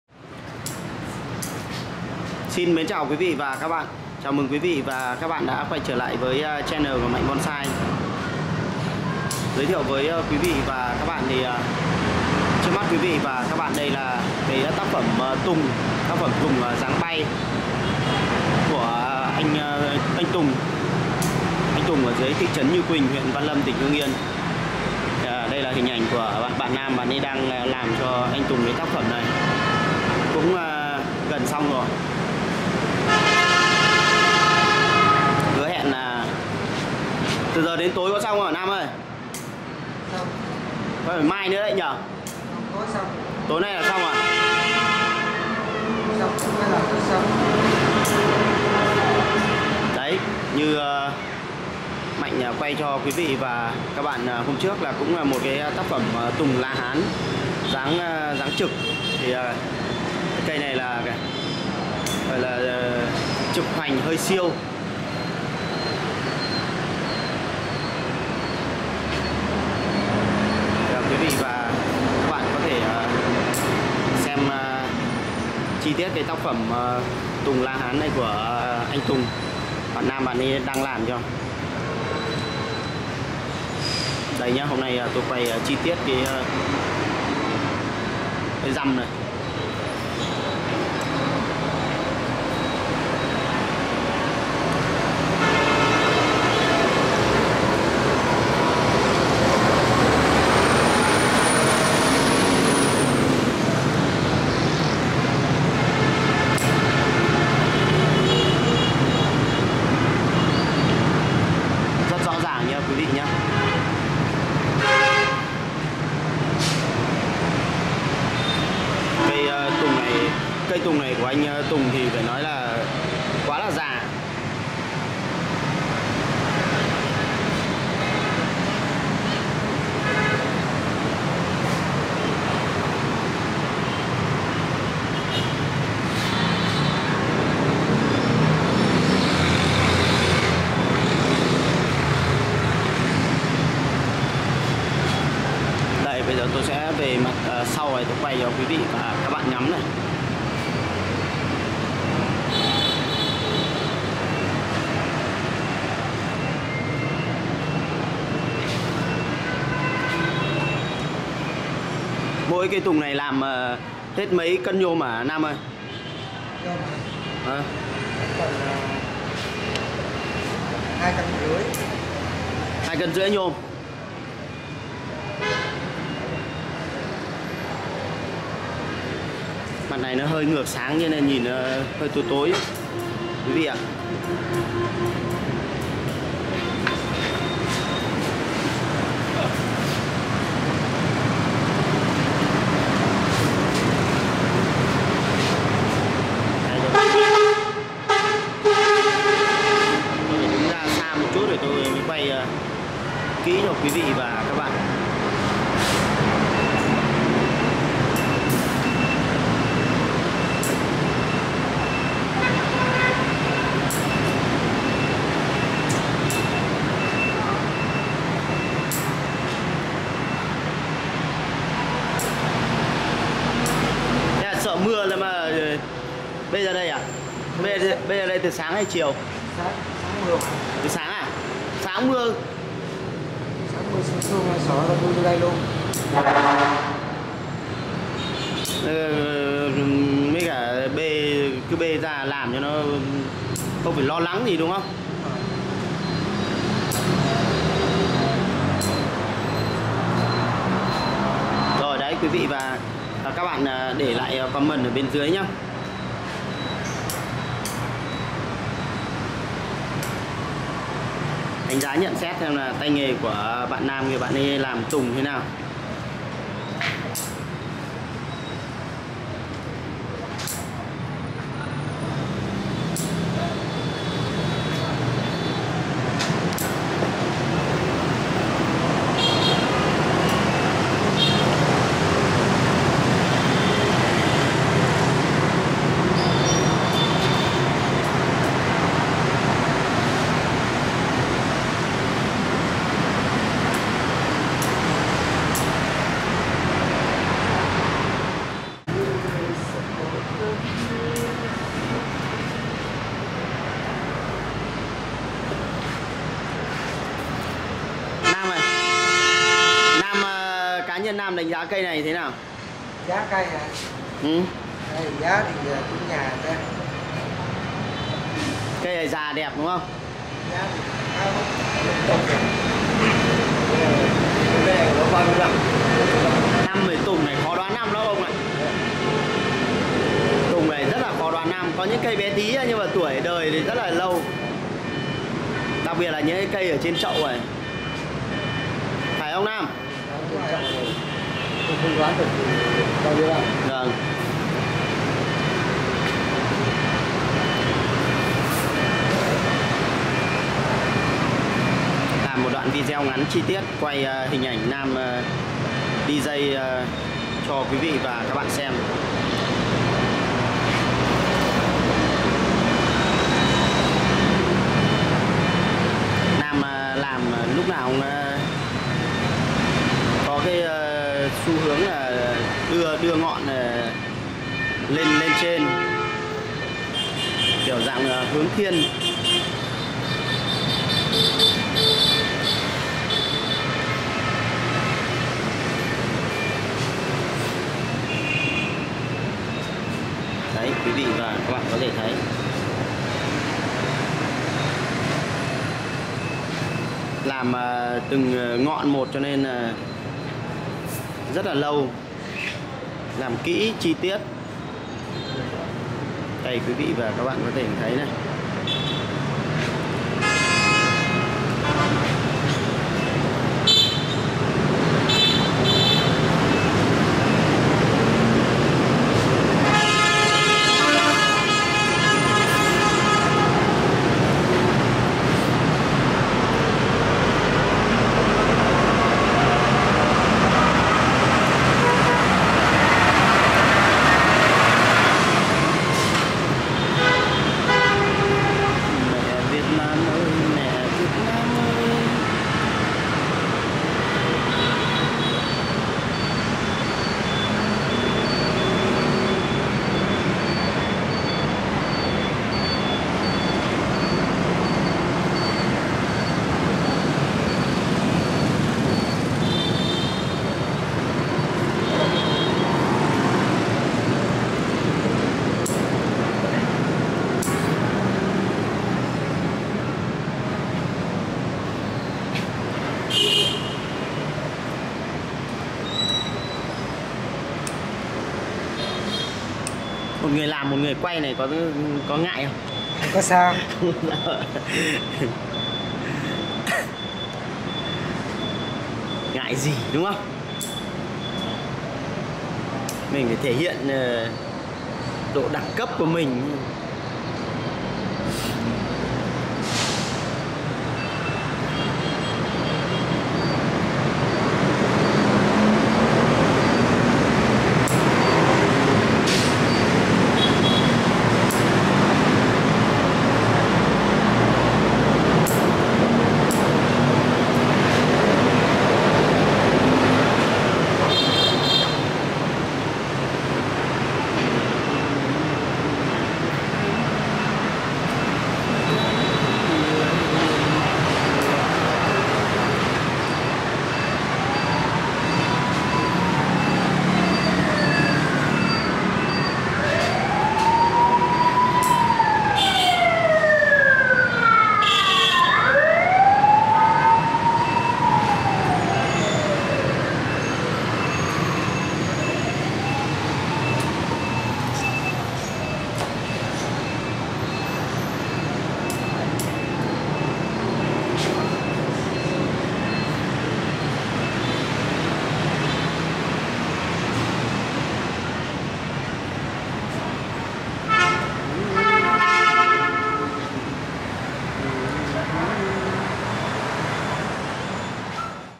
Anh Nam thợ Quy Nhơn Bình Định làm đẹp cây Tùng La Hán cho anh Tùng
bonsai